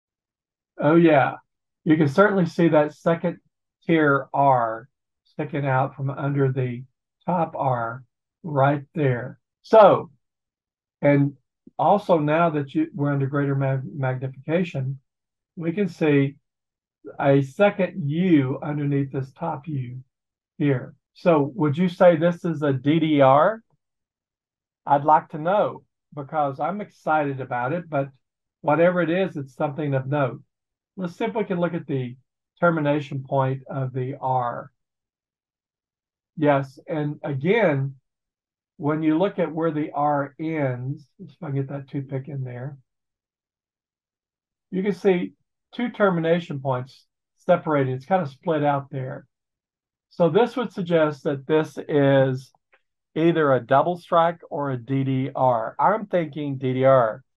ECC Short #157 Double Strike or DDR
In this video Ron shows and talks about a Lincoln Penny.  In this video he makes a case for a DDR but asks you if it could actually be a double strike...you decide.